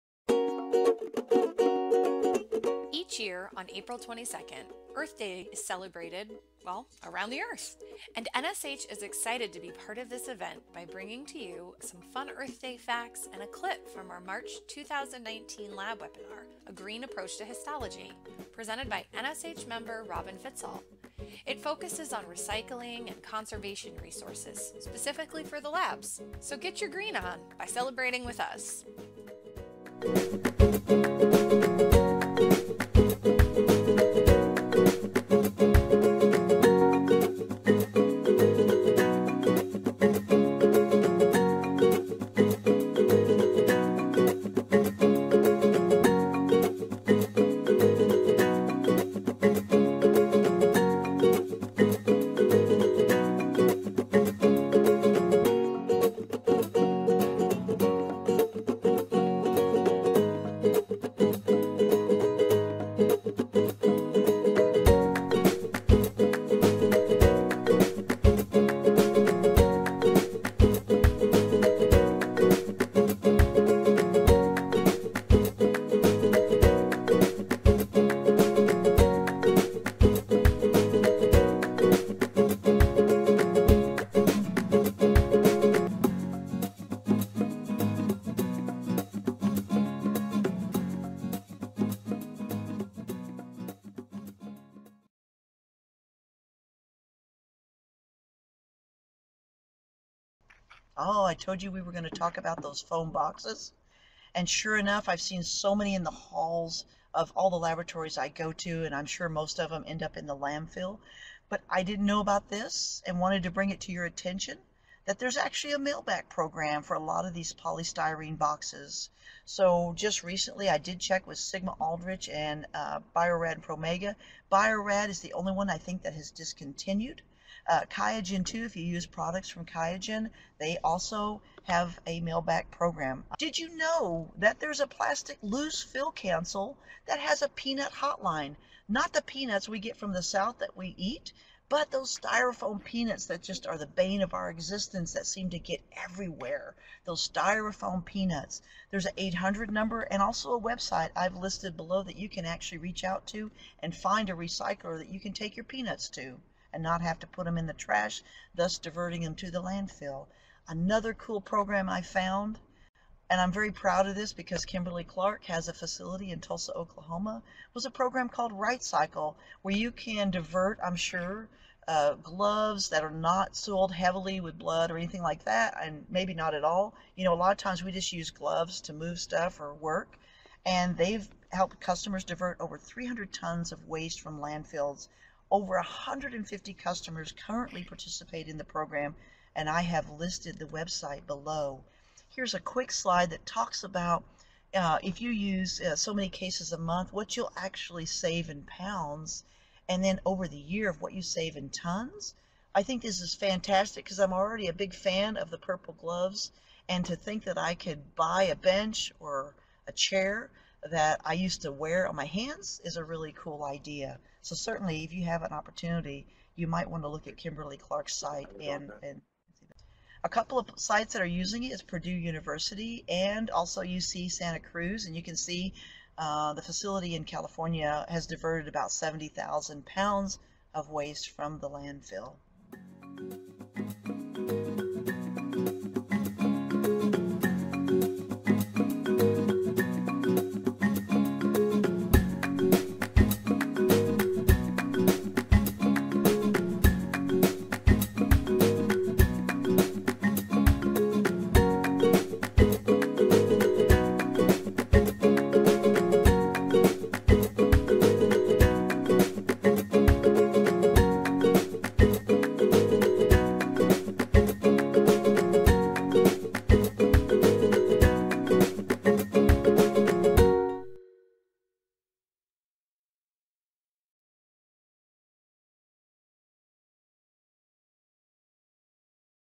NSH Earth Day Celebration - 2019
NSH is excited to celebrate Earth Day, 2019!  This short video features some interesting Earth Day facts and a clip from our recent Laboratory Webinar, A Green Approach to Histology.  If you are a NSH member, don't forget to click here to access the quiz for this event worth one CEU